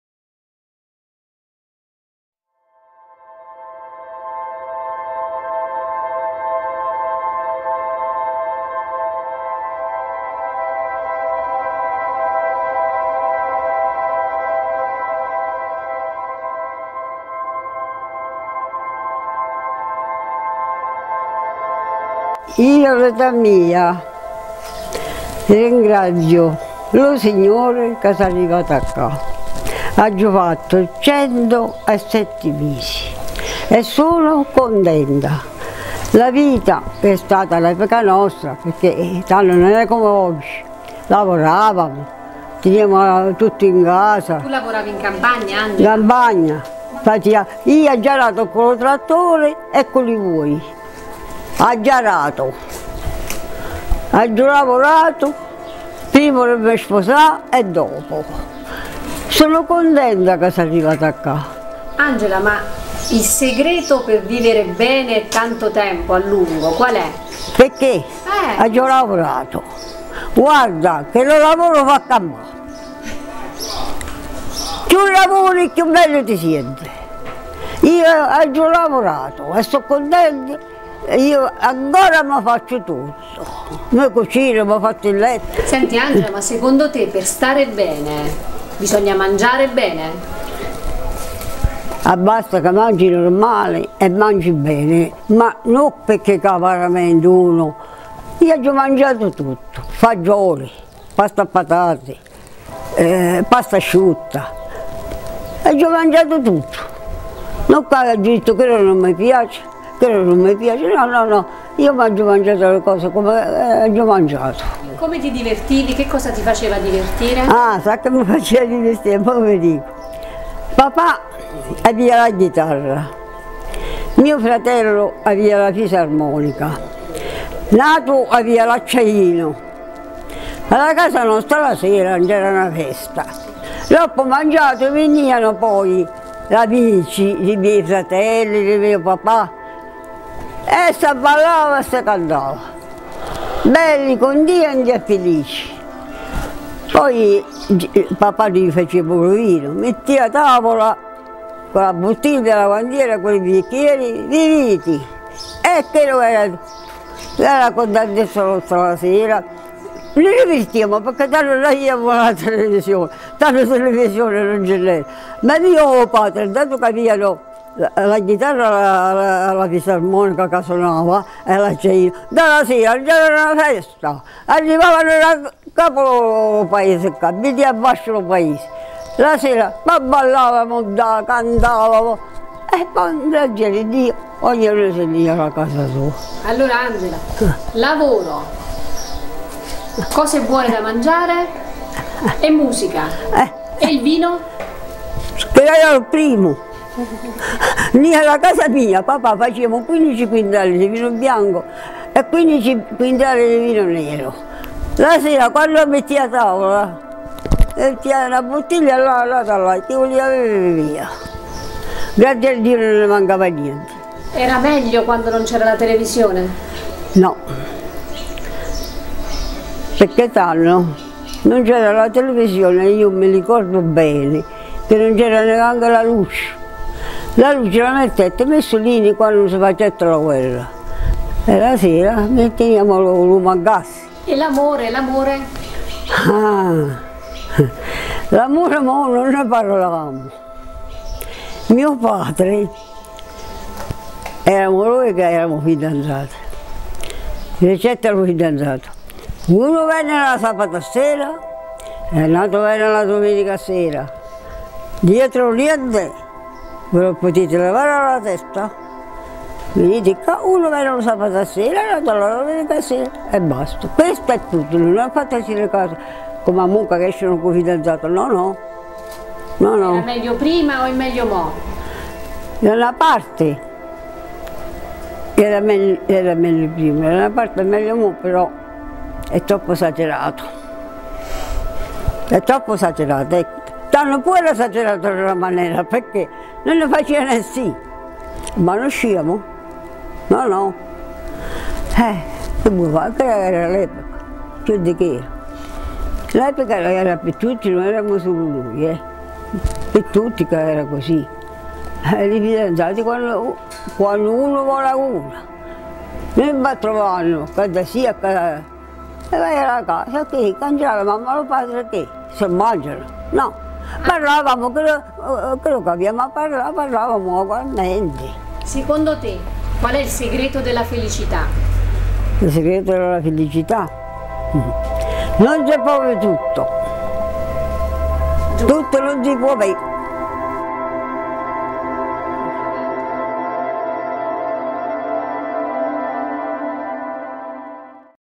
LONGEVI 2021. Angela Mollo, 101 anni
In Italia i centenari aumentano, i dati ISTAT lo confermano. Un’aspettativa di vita che, per quanto ci renda un Paese “vecchio”, ci riconosce una longevità di cui andar fieri.
Convinti che un gran merito vada all’alimentazione ed alla Dieta Mediterranea, lo shop online Masilicò (marchio di prodotti cilentani di pregio) in collaborazione con la chef Giovanna Voria dell’Agriturismo Corbella – ideatrice del progetto - ed il blogzine Rosmarinonews.it - fondato dalla giornalista gastronomica Antonella Petitti - hanno dato vita a “Longevi 2021”.
In attesa che venga svelato il progetto, ecco un’anticipazione attraverso le videointerviste dei protagonisti.

- Angela Mollo, 101 anni
Cicerale (SA)